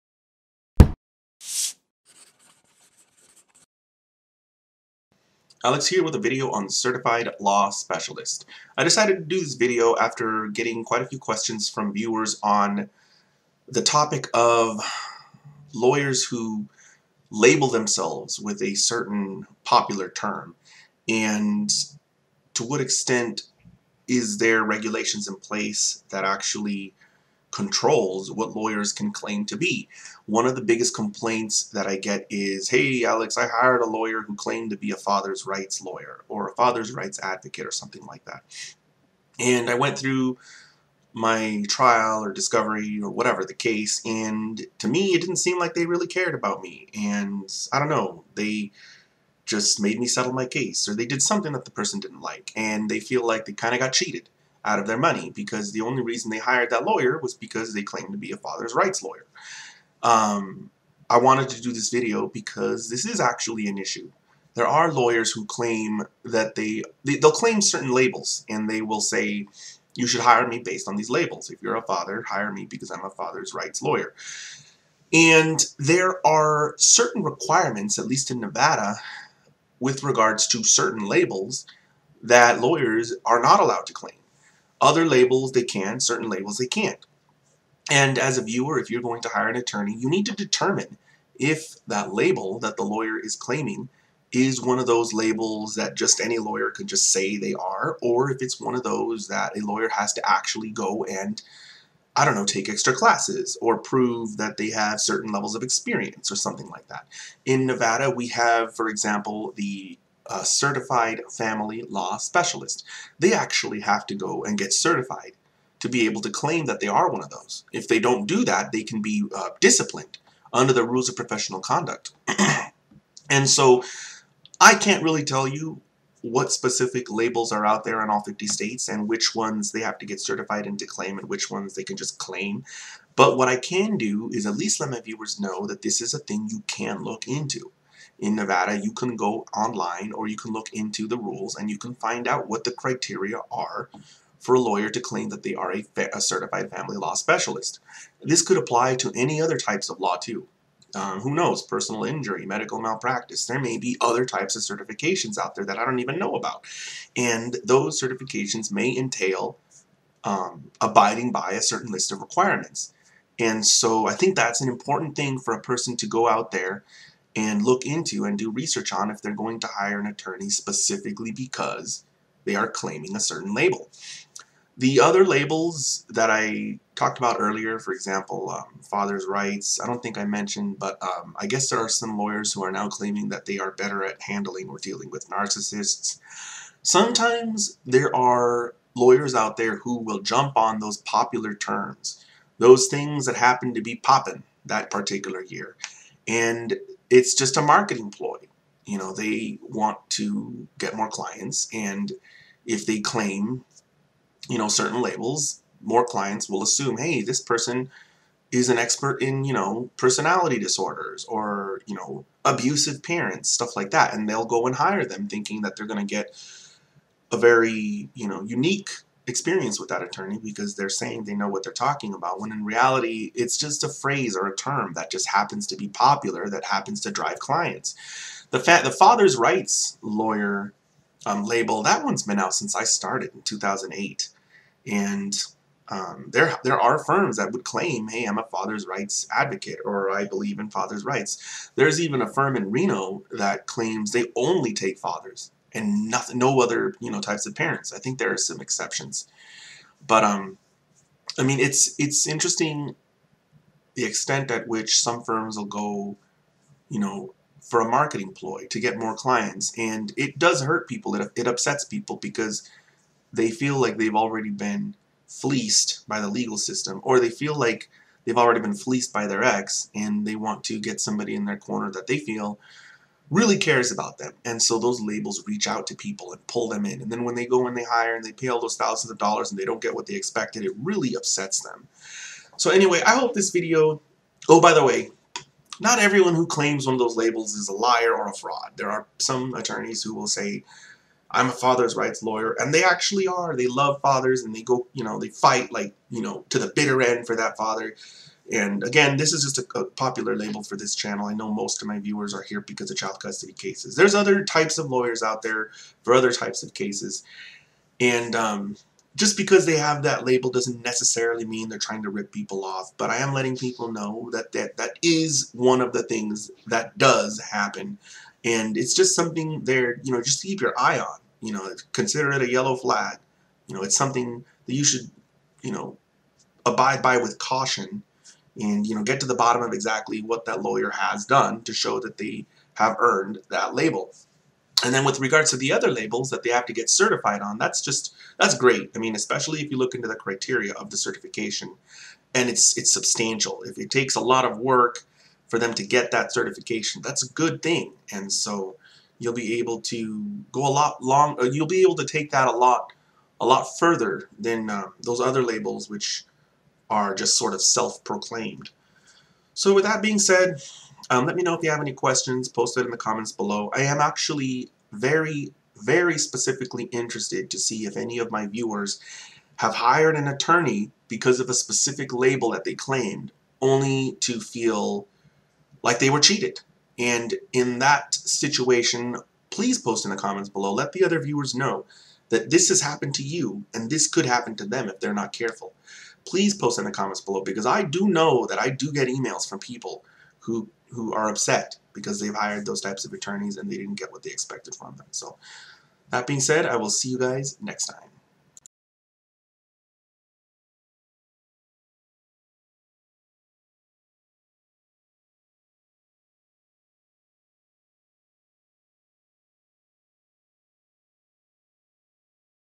Legal Nuts And Bolts: Certified Law Specialists
I cover the concept of "certified" law specialists, and the troubles associated with lawyers who adopt labels like "father's rights" in order to market themselves.

I'm not an attorney. Nothing in this video is intended to constitute legal advice.